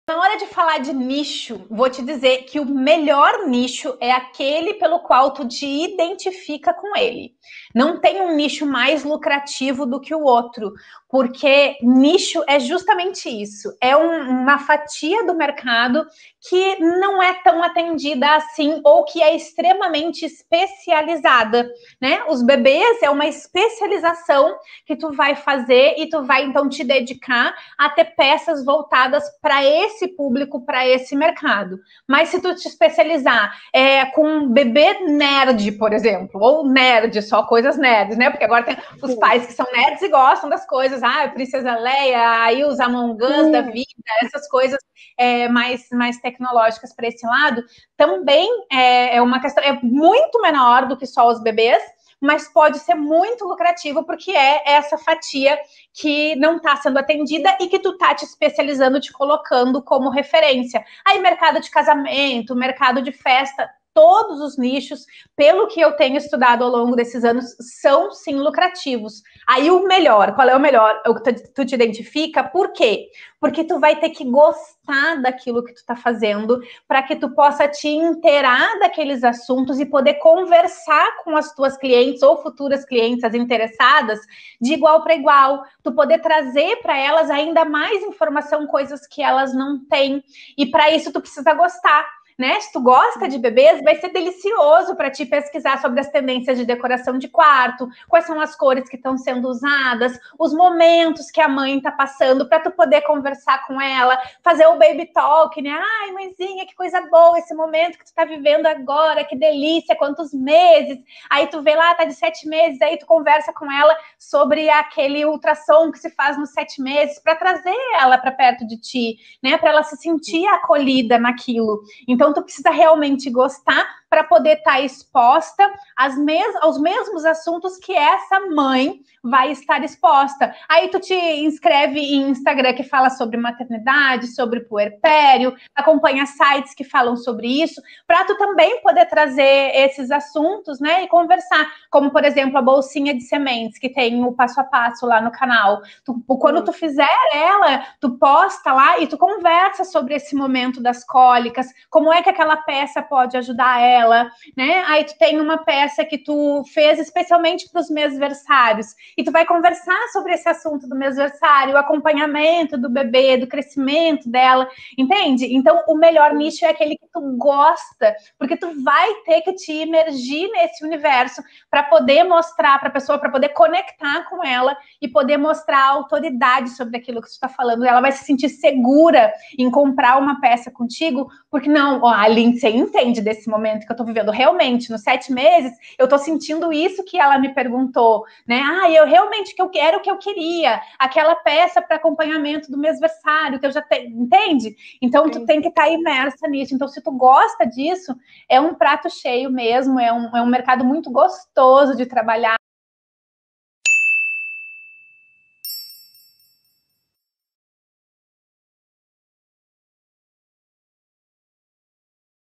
O MELHOR NICHO EM AMIGURUMI$ | Mariana Fontoura
Ah, o nicho! Ele não é indispensável, mas é de uma ajuda que não dá nem pra mensurar e isso muitas vezes tira o sono pra descobrir se o nicho escolhido é mesmo o melhor...

Eu trouxe esse trechinho da mentoria da Lindsei (bônus extraordinário da turma de Fev/21) porque explica direitinho a minha visão em relação à isso.

Escolher um nicho pra se trabalhar é tipo escolher marido, se casar por dinheiro a coisa foge do propósito e não vai pra frente. Um dos dois acaba infeliz e as chances são que vai ser tu.

Então escolher um nicho pensando se ele é lucrativo ou não sem levar em conta o fator "o que EU gosto de fazer/viver" provavelmente vai ser insustentável, insuportável ao longo do tempo.

Afinal nós somos o nosso ateliê e fica muito mais fácil se envolver com algo que tu te identifique, às peças, os conteúdos fluem e o fazer dinheiro com os Amigurumi$ 🤑 vem de forma mais natural.

E tu, já escolheu teu nicho?
Ou ainda tá em dúvida?